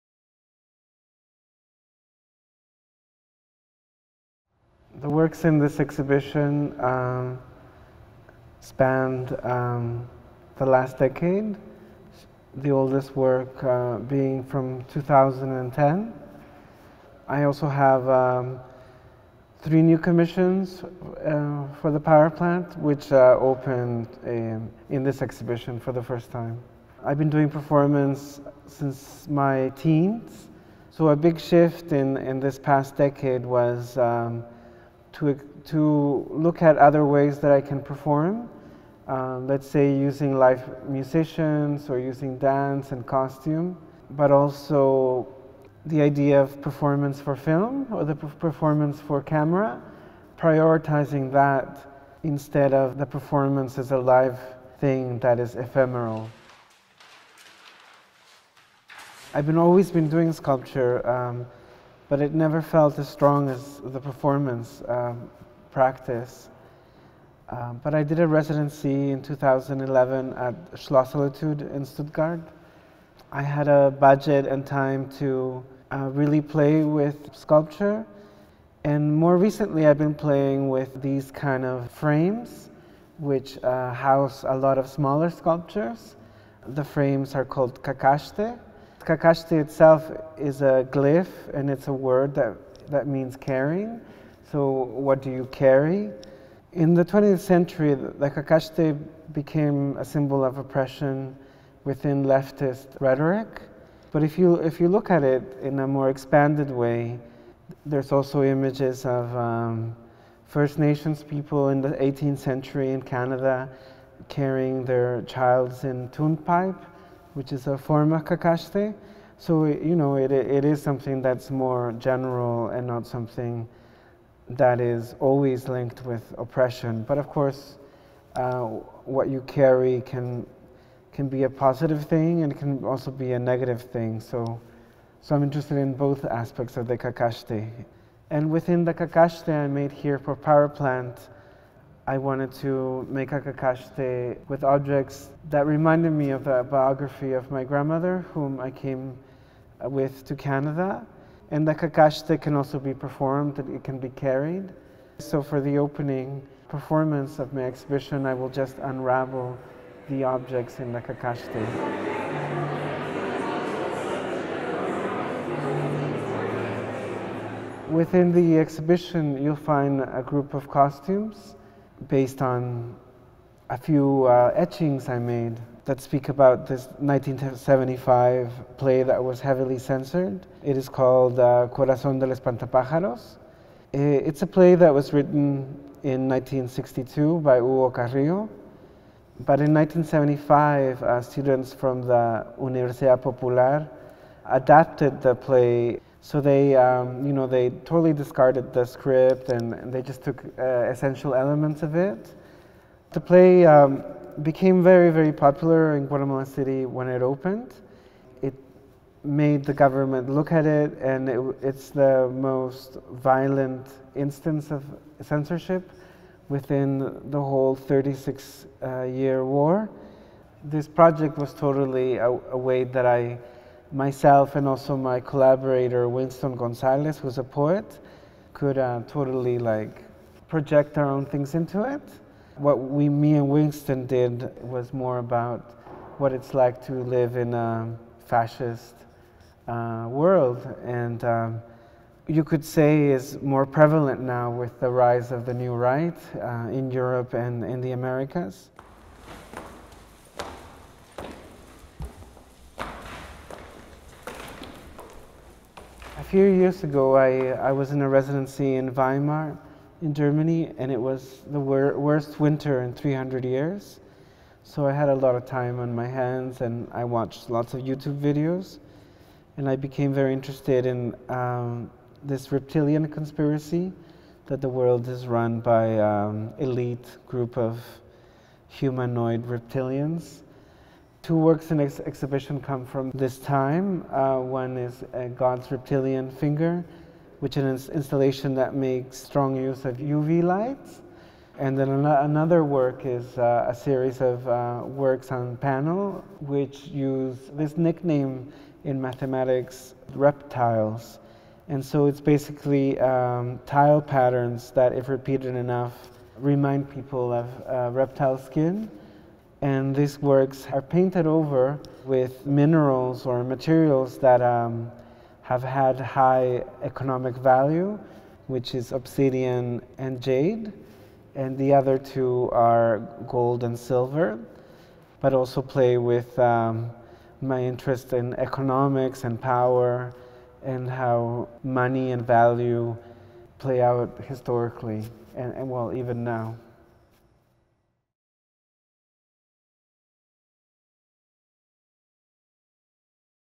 The Power Plant: Interview with Naufus Ramírez-Figueroa
Canadian-Guatemalan artist Naufus Ramírez-Figueroa works across media, using forms including sculpture and performance that draw on literature, Latin American history, folklore and childhood memories.

The exhibition encompasses works from the past decade that reveal the range of Ramírez-Figueroa’s preoccupations, from conspiracy theories to bird song and Guatemala’s architectural history.